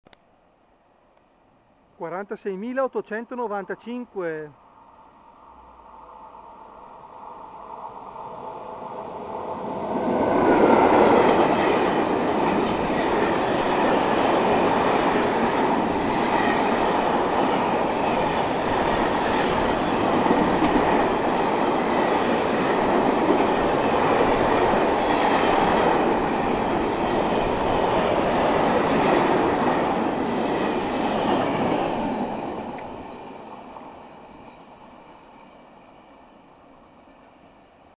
MRI 46895
PER VEDERE MEGLIO IL VIDEO, CLICCATE SU "HQ" SOTTO AL VIDEO.

Treno merci MRI (Merci Rapido Internazionale) 46895 Villanova d'Asti-Villa Opicina in transito a Ponte S.Marco-Calcinato.